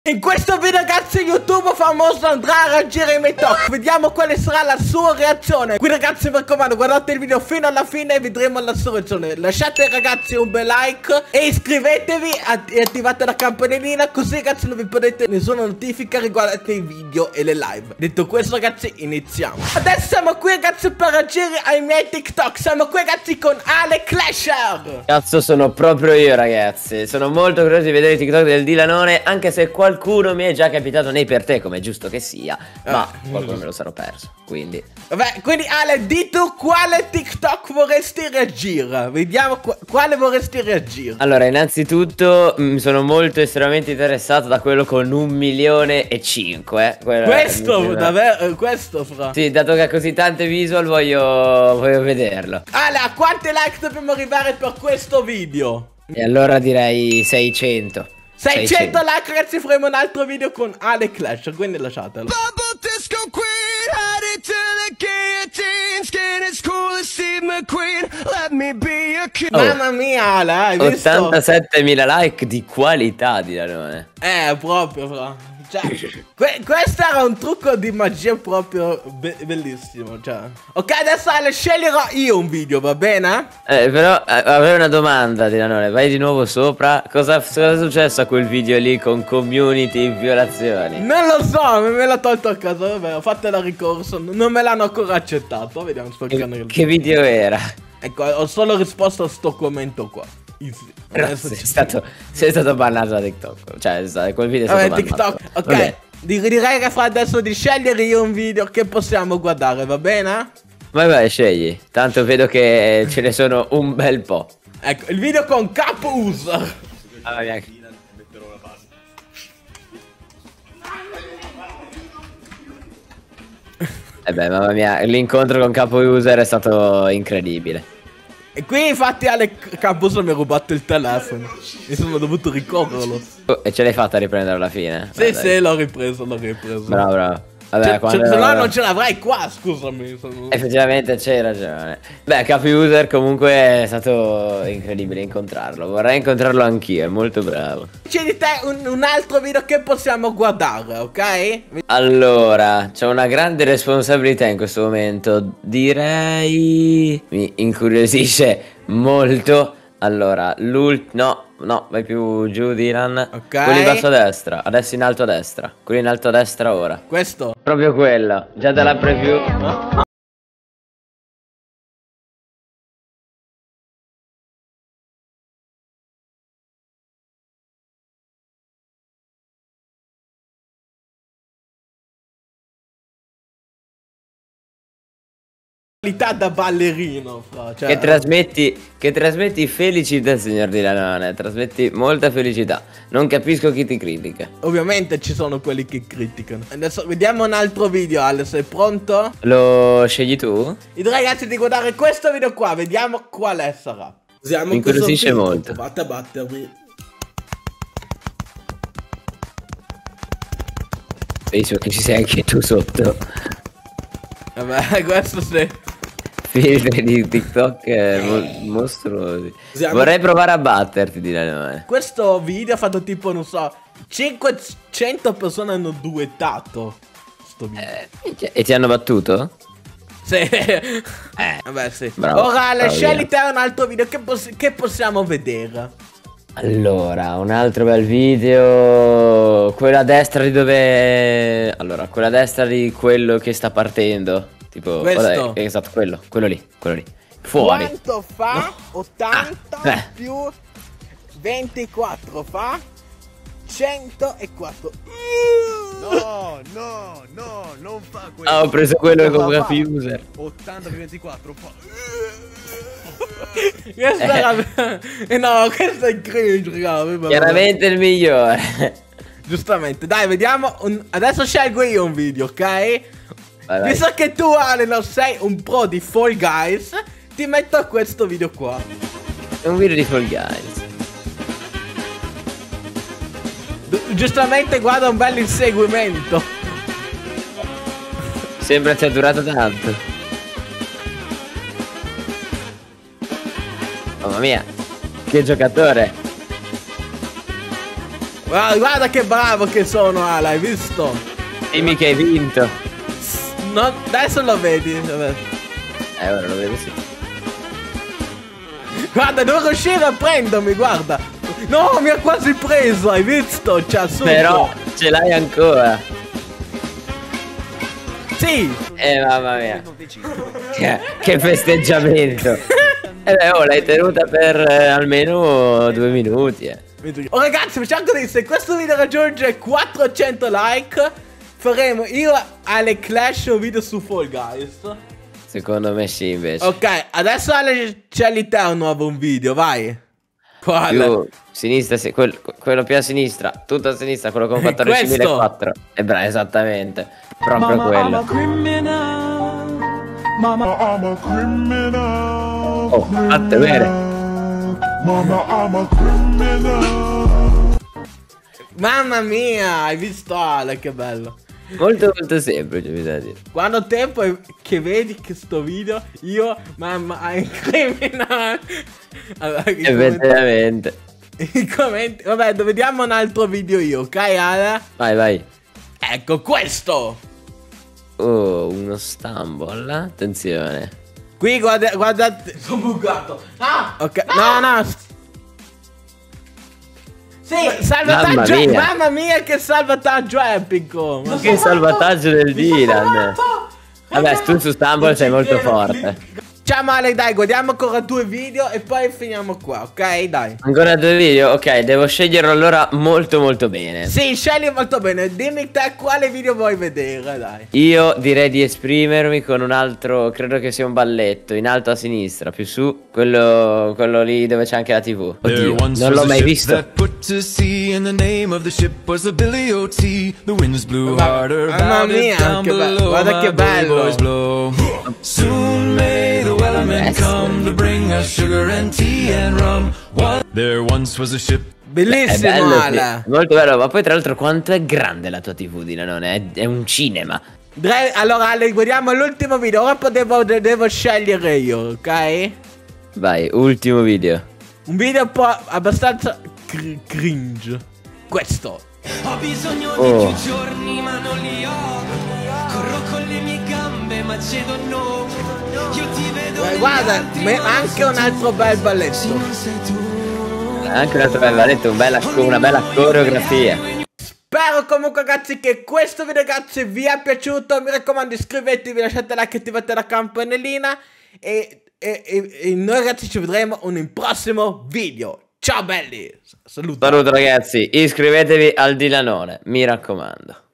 ALECLASHER REAGISCE AI MIEI TIKTOK🥵 *CRINGE*
CIAO RAGAZZI IN QUESTO VIDEO  @ALECLASHER   REAGISCE AI MIEI TIKTOK  VEDIAMO COSA NE PENSA TROVATE LA PAROLA SEGRETA PER VENIRE SALUTATI NEL PROSSIMO VIDEO